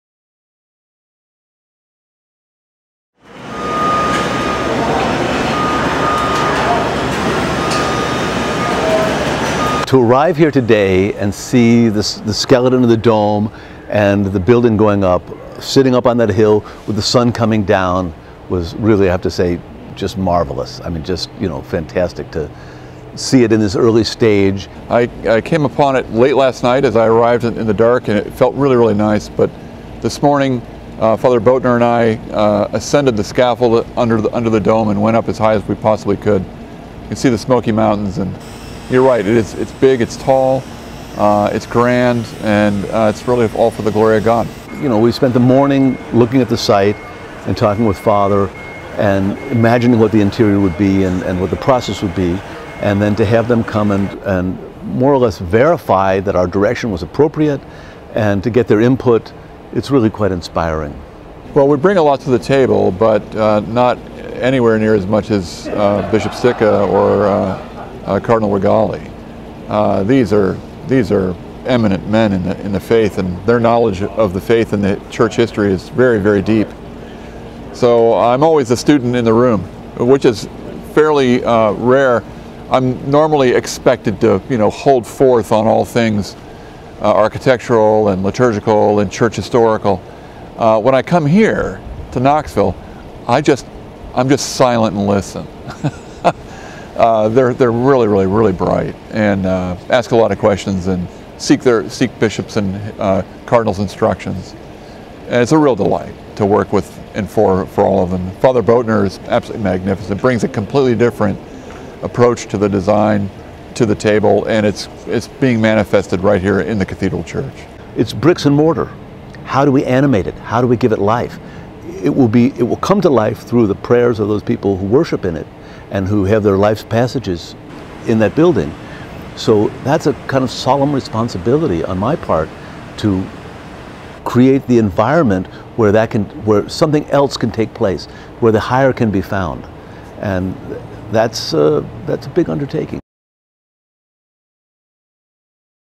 Architectural Artists
Jeff Greene with Evergreene Architectural Arts and James McCrery with McCrery Architects came to Sacred Heart last Tuesday to check on the progress of the cathedral.